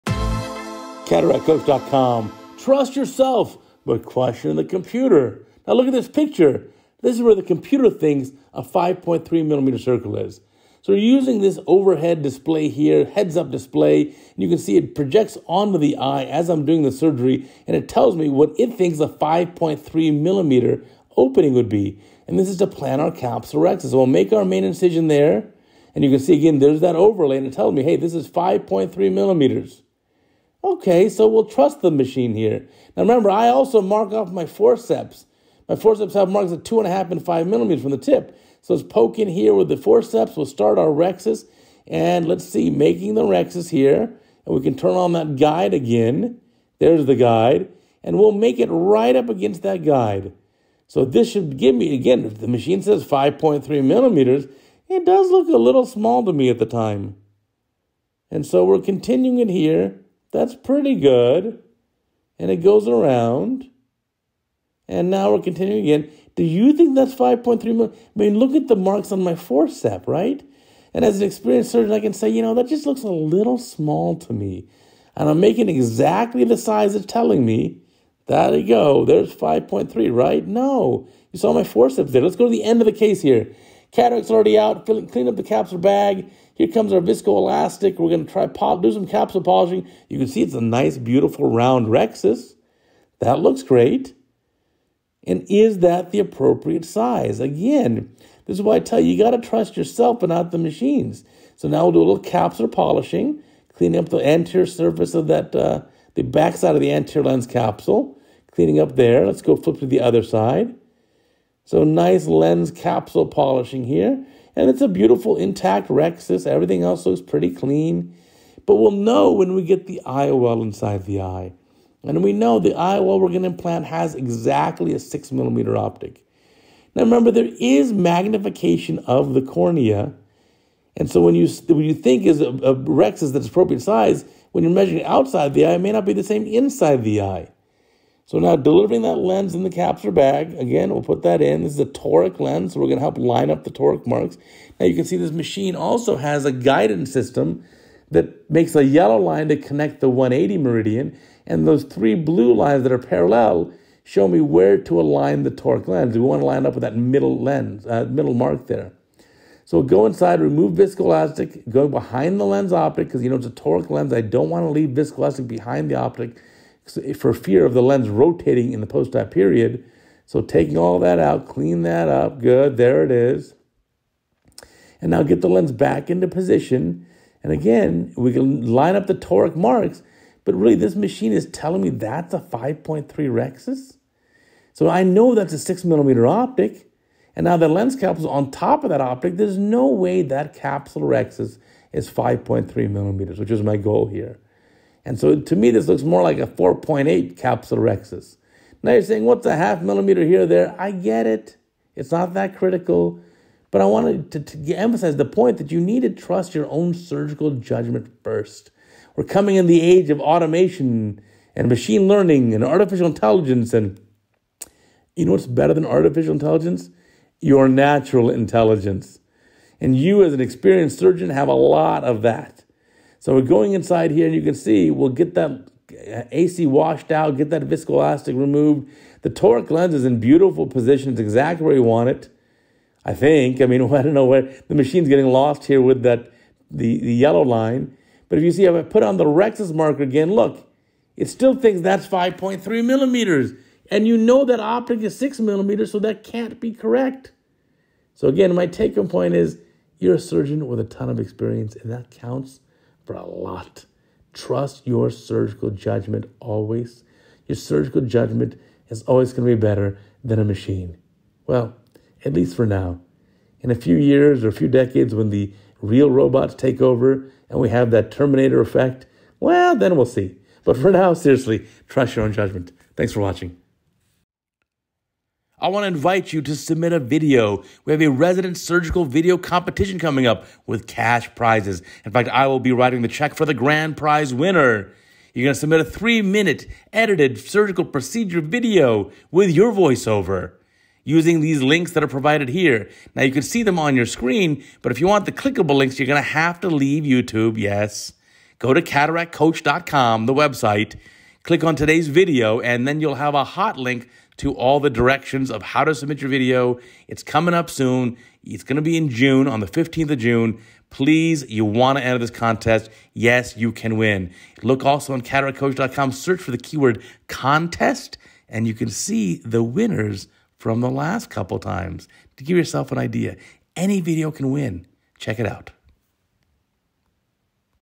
CataractCoach™1830: trust yourself but question the computer
I recently performed a cataract surgery case using a heads-up electronic display where the computer would overlay a precise circle to serve as a guide for capsulorhexis creation. I chose a diameter of 5.3mm and when I make the capsulorhexis according to this guide, it seemed rather small, perhaps 4.8 mm or even smaller. At the end of the case you can see that there is a 6.0 mm optic in the eye but the computer still thinks that the overlay of the blue guide is 5.3 mm -- and of course that does not seem right. I'm sure these devices will get better with time and perhaps with more input data, but even then, trust your own judgment the most.